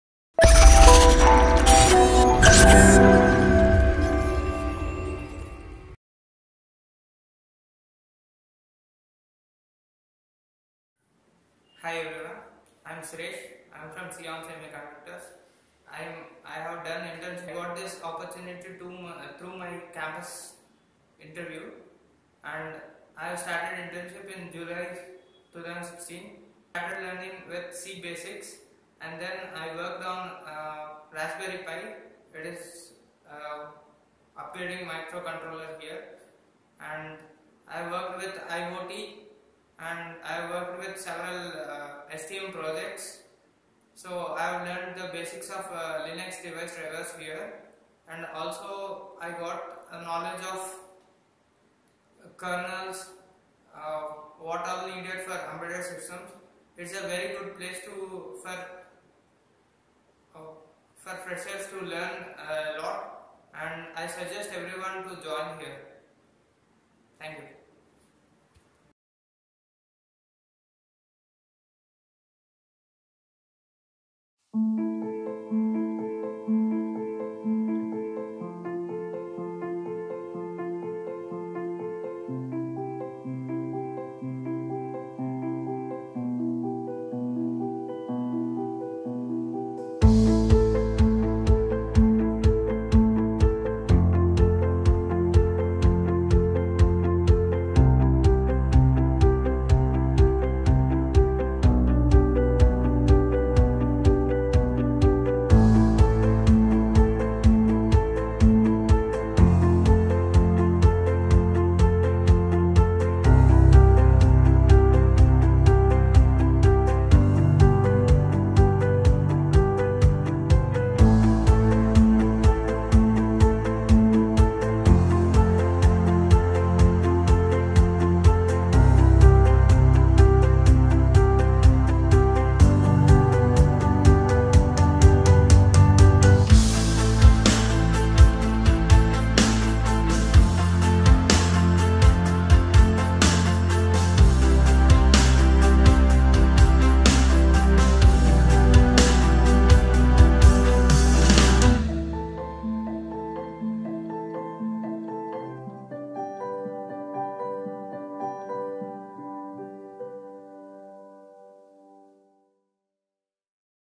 Video Testimonial : SION Embedded Internship : Mr Suresh
Video Testimonial from our intern Mr. Suresh, who had done B.Tech from an Engineering college located in Vijayawada, Hyderabad.

He opted Embedded systems internship with SION, and in 6months of time, he got opportunity to work on various cutting edge platforms like Arduino, Raspberry Pi3 and STM ARM Boards, and thus acquired sound practical skills in ARM, Embedded C, IOT and RTOS.

This practical experience helped him, getting several corporate offers from SION and other MNCs in Bangalore. All the best Suresh ! Keep going !!